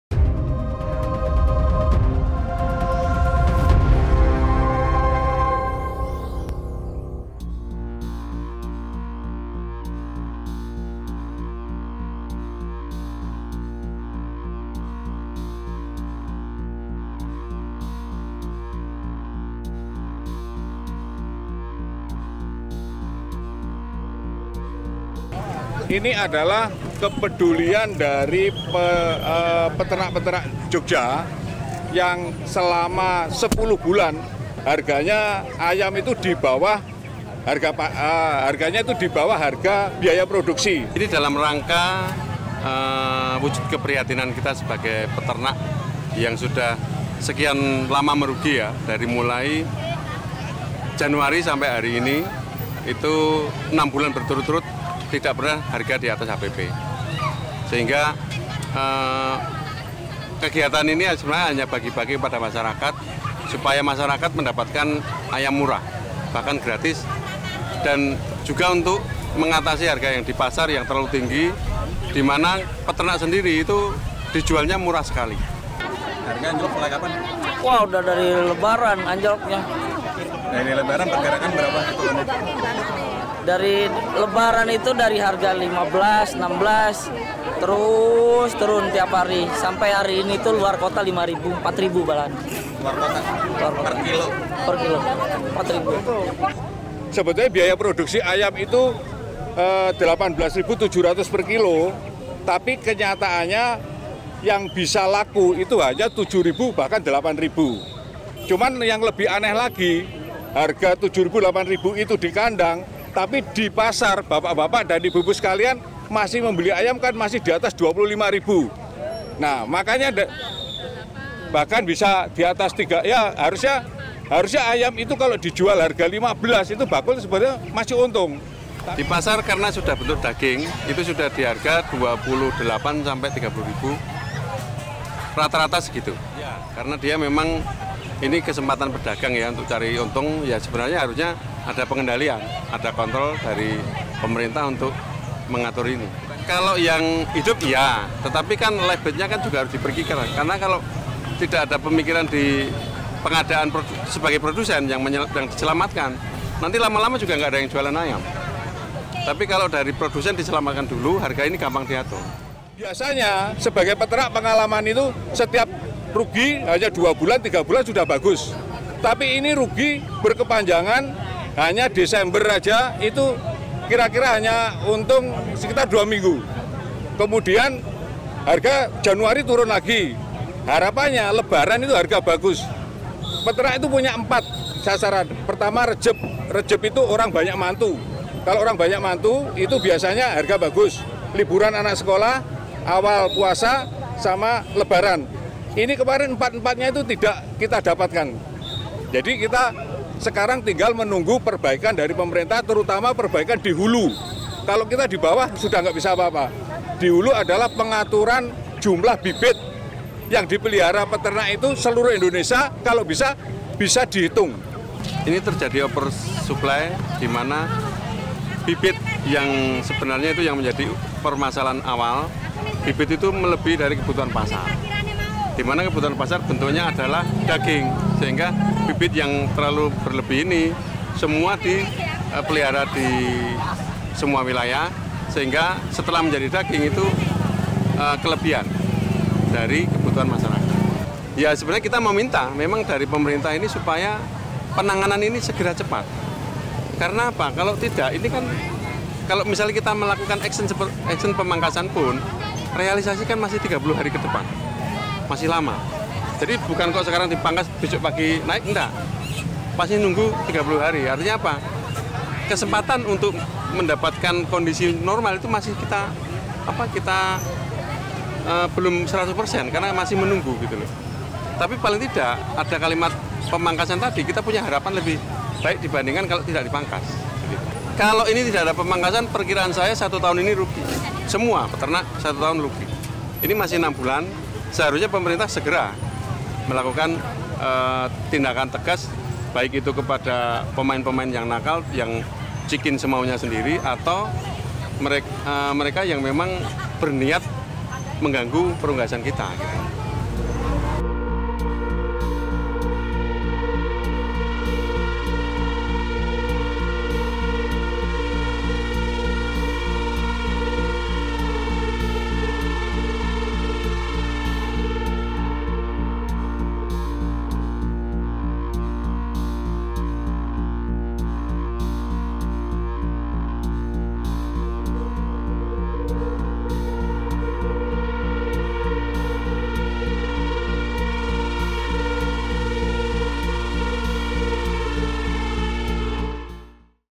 Peternak Broiler Bagikan Ayam Gratis
Peternak broiler (ayam pedaging) seringkali mengalami harga jual ayam hidup (livebird) yang berada di bawah HPP (harga pokok produksi) mereka. Pada Juni 2019 lalu, para peternak broiler sangat terpukul dengan rendahnya harga livebird, pasalnya livebird mencapai Rp 8 - 9 ribu per kg. Mereka yang tengah dirundung pilu itu memutuskan untuk membagikan ayam-ayam mereka ke pada masyarakat sebagai salah satu aksi protes dan agar masyarakat sebagai konsumen mendapat keadilan, lantaran di pasar harga daging ayam sendiri, normal. Simak liputan selengkapnya.